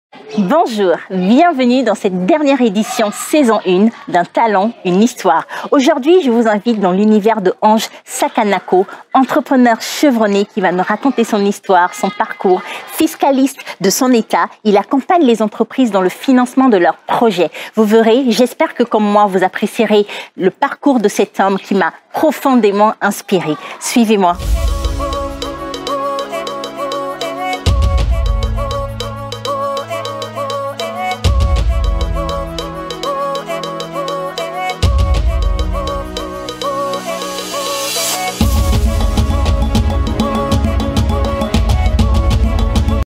TEASER EPISODE 13 ( Ange SAKANOKO )
Pour clôturer cette première saison en beauté. Un talent, Une histoire est allé à la rencontre d'un personnage aussi atypique que son parcours, son nom Ange SAKANOKO.

🚨 Rendez-vous ce Mardi 16 Novembre à 18h00 pour découvrir son Histoire   🎬.

Chapitres
0:00 Intro
0:15 Presentation
0:30 Fin

Conception, présentation : Vanessa TSOUMA
Montage : Bedy OBAMBI
Réalisation : Julien Biatchon et Fabrice Pandi
Animation : Jalade Mongo
Stylisme : Vanessa TSOUMA